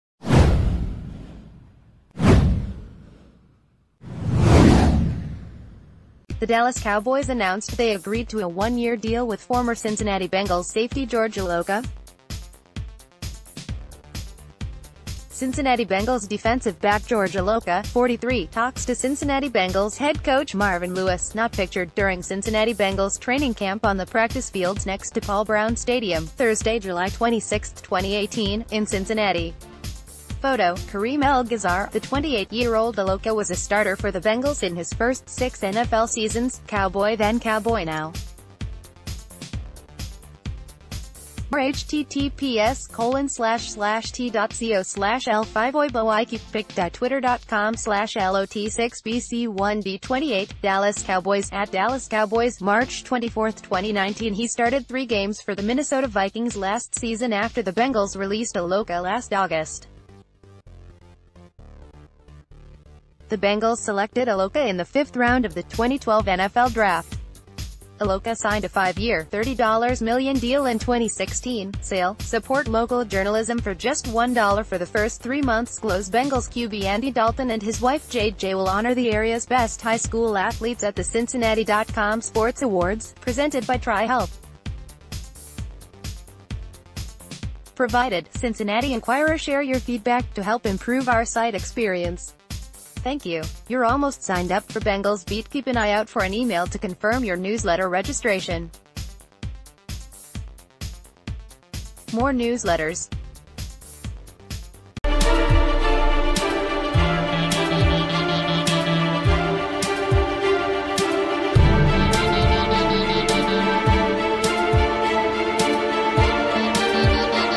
Former Bengals safety George Iloka signs with Dallas Cowboys
The Dallas Cowboys announced they agreed to a one-year deal with former Cincinnati Bengals safety George Iloka. Cincinnati Bengals defensive back George Iloka (43) talks to Cincinnati Bengals head coach Marvin Lewis (not pictured) during Cincinnati Bengals training camp on the practice fields next to Paul Brown Stadium, Thursday, July 26, 2018, in Cincinnati.  (Photo: Kareem Elgazzar)The 28-year-old Iloka was a starter for the Bengals in his first six NFL seasons. Cowboy then Cowboy now Blessed...